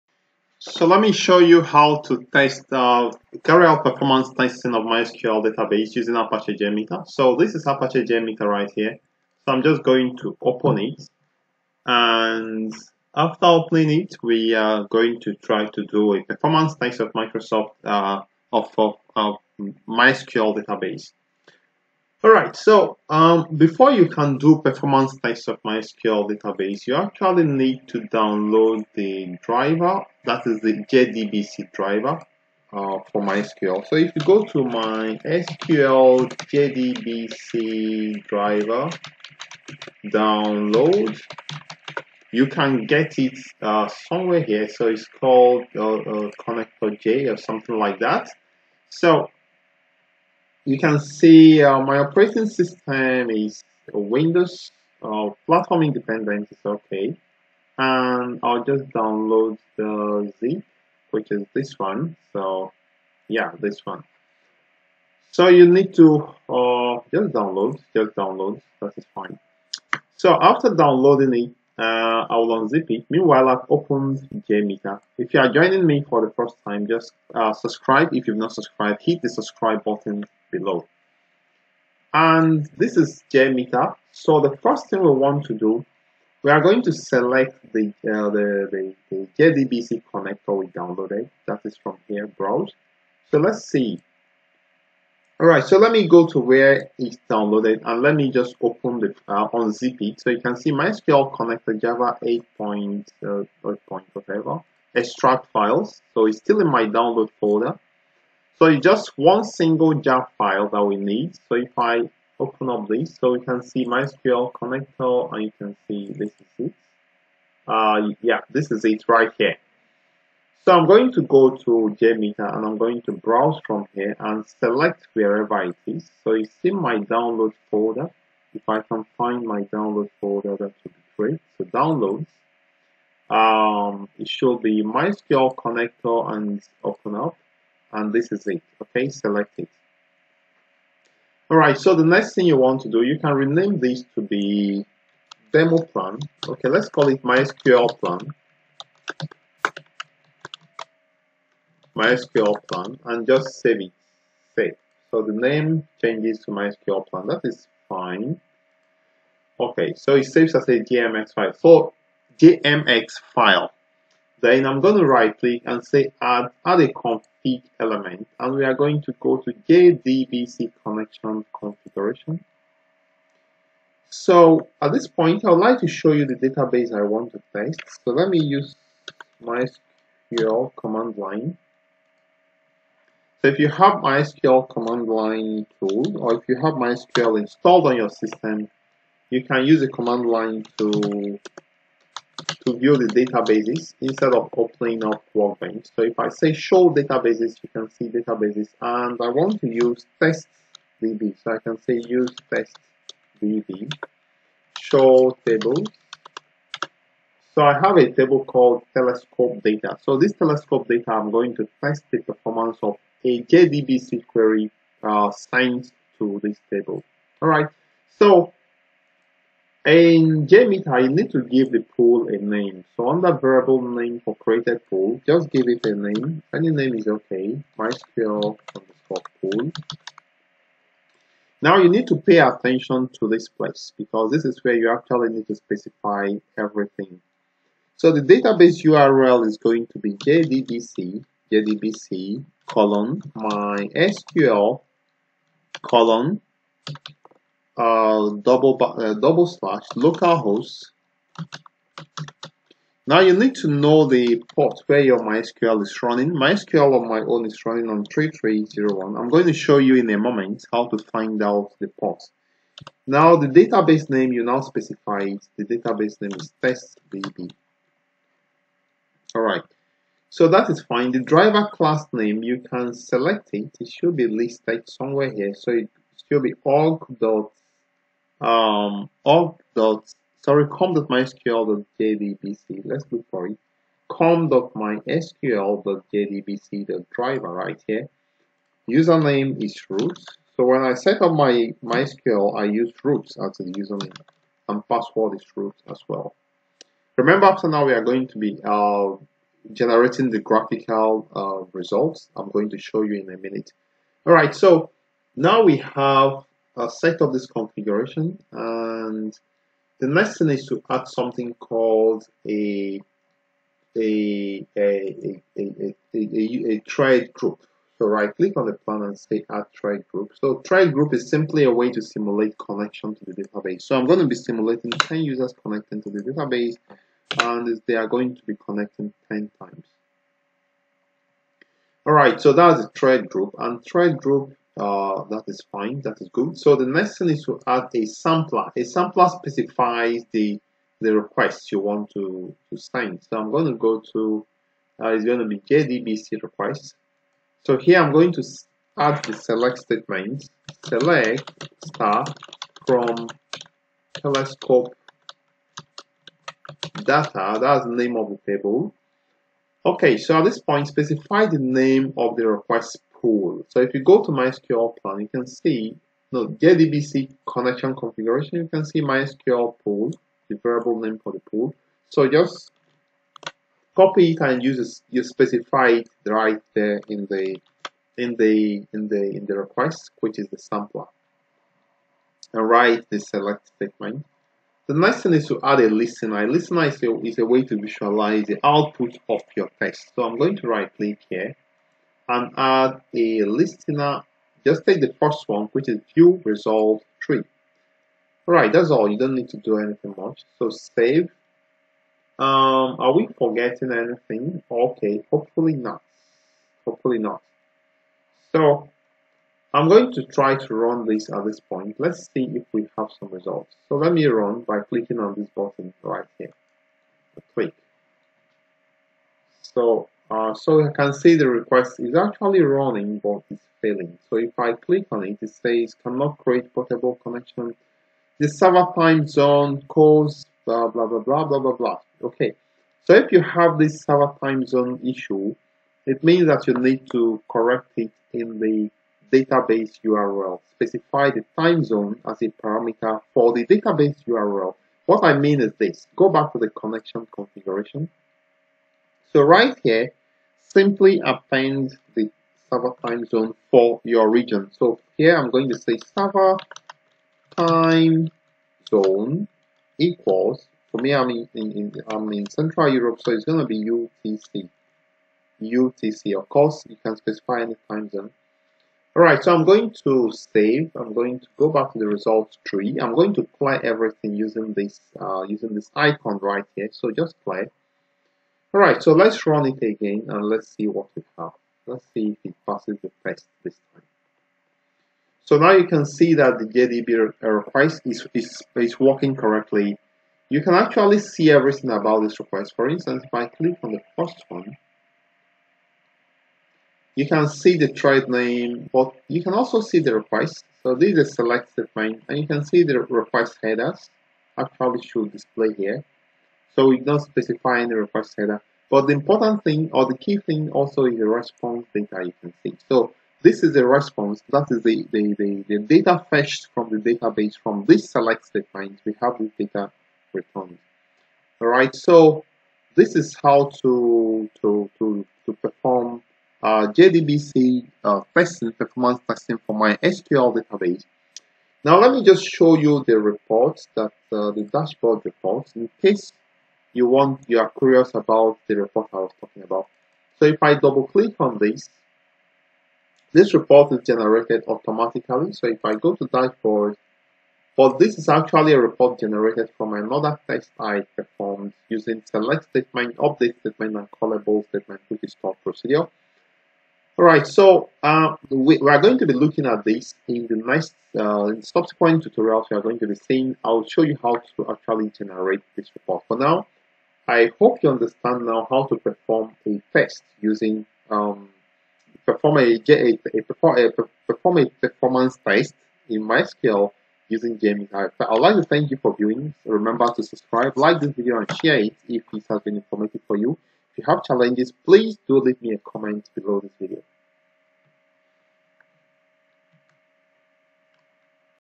How to Test MySQL Database Server Using JMeter(Performance Testing)
This video explains how to carry out performance testing on MySQL Database Server Using JMeter

JMeter Tutorials